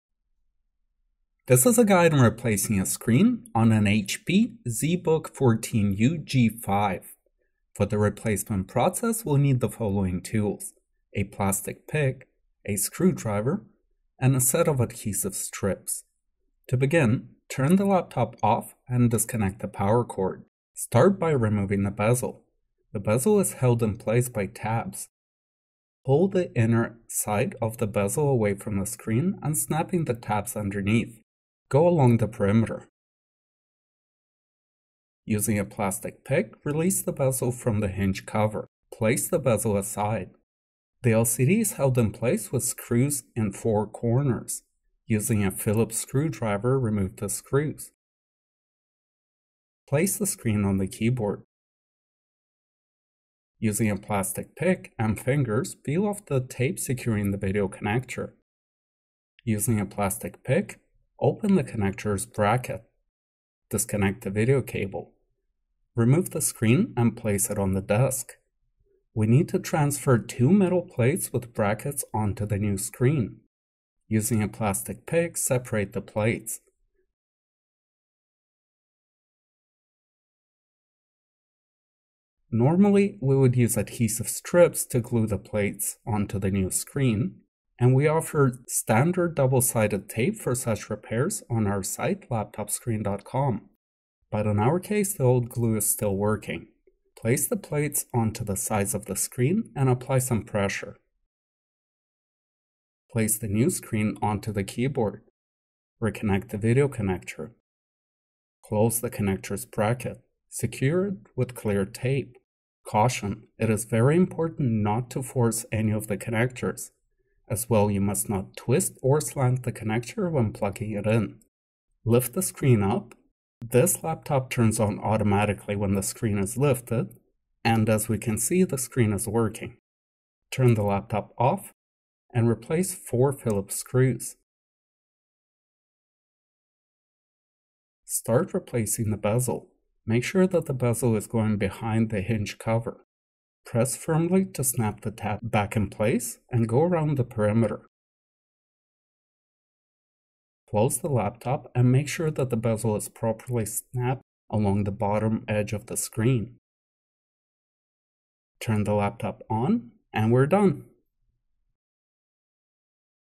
How to replace LCD Screen on HP Zbook 14U G5
This is a guide on replacing a screen on HP Zbook 14U G5

For the replacing process we will need the following tools:
- a plastic pick
- a screwdriver
- a set of adhesive strips

To begin turn the laptop off and disconnect the power cord.

Start removing the bezel. The bezel is held in place by tabs.
Pull the inner side of the bezel away from the screen unsnapping the tabs
underneath. Go along the perimeter. Using plastic pick release the bezel
from the hinge cover. Place the bezel aside.

The LCD screen is held in place with screws in four corners. Using Phillips
screwdriver remove the screws. Place the screen on the keyboard.
Using plastic pick and fingers peel off the tape securing the video connector.
Using plastic pick open the connector's bracket. Disconnect the video cable.
Remove the screen and place it on the desk.

We need to transfer two metal plates with brackets onto the new screen.
Using plastic pick separate the plates. Normally we would use adhesive strips
to glue the plates onto the new screen.

We offer standard double-sided tape
for such repairs on our site: laptopscreen.com

But in our case the glue is still working.
Place the plates onto the sides of the screen and apply some pressure.

Place the new screen onto the keyboard. Reconnect the video connector. Close
the connector's bracket. Secure it with a clear tape. Lift the screen up.

As we can see the screen is working!

Turn the laptop off. Replace 4 Phillips screws.

Start replacing the bezel. Make sure that bezel is going behind the hinge cover.
Press firmly to snap the tabs back in place and go around the perimeter.
Close the laptop and make sure that the bezel is properly snapped along
the bottom of the screen.

Turn the laptop on, and we are done!